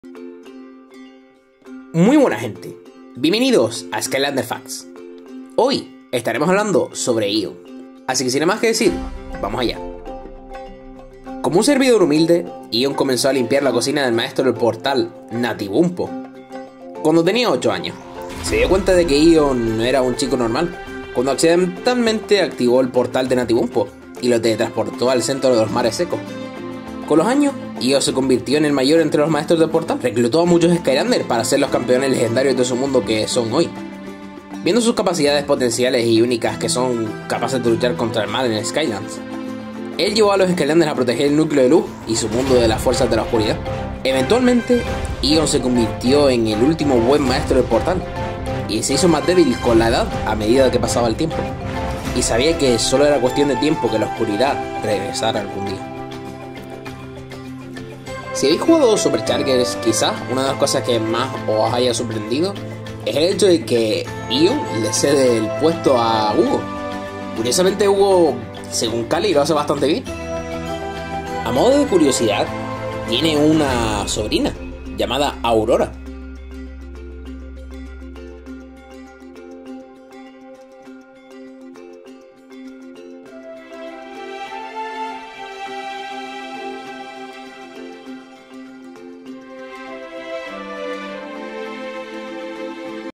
Skylanders Facts! | EON Y SU HISTORIA
Muy buenas gente, bienvenidos a Skylanders Facts!, hoy es el turno de el mayor Maestros del Portal de todos los tiempos, Eon, es un tema poco tratado y que desde luego tiene mas cosas curiosas de lo que esperaríamos...

🔥🎩𝐬𝐢 𝐥𝐥𝐞𝐠𝐚𝐬𝐭𝐞 𝐚𝐪𝐮í 𝐞𝐫𝐞𝐬 𝐨𝐟𝐢𝐜𝐢𝐚𝐥𝐦𝐞𝐧𝐭𝐞 𝐮𝐧 𝐞𝐫𝐮𝐩𝐭𝐨𝐫𝐬𝐨𝐦𝐛𝐫𝐞𝐫𝐨🎩🔥